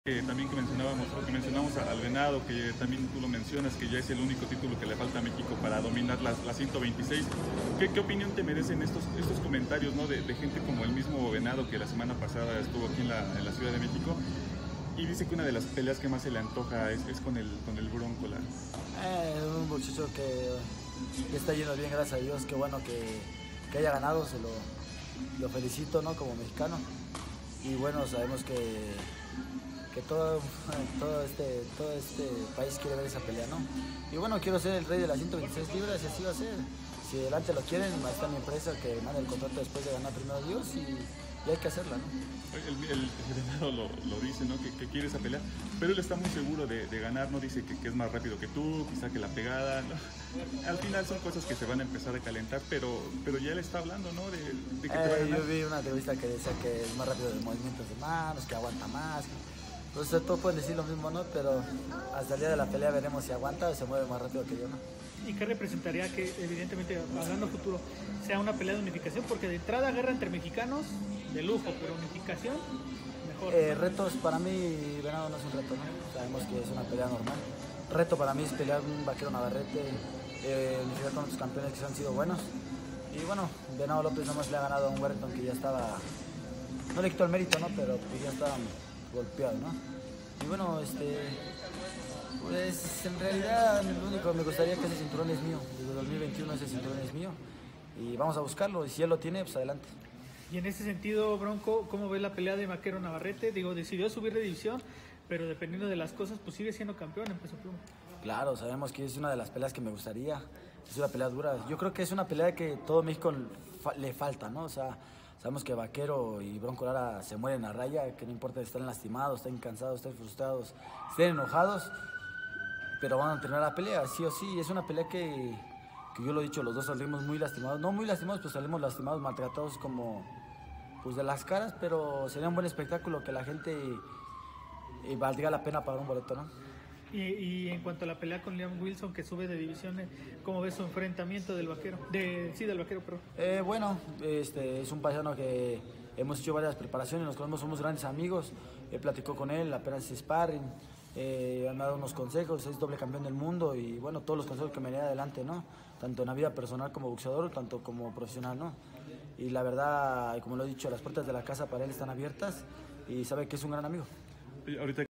Habla BRONCO LARA: "Para mí, VENADO LÓPEZ no es un reto, pero el VAQUERO NAVARRETE sí”
MAURICIO "BRONCO" LARA asegura que LUIS ALBERTO "VENADO" LÓPEZ no representa un reto, pues le ganó a un JOSH WARRINGTON ya golpeado.

Pero piensa que para él, EMANUEL "VAQUERO" NAVARRETE sí representa un reto para su carrera.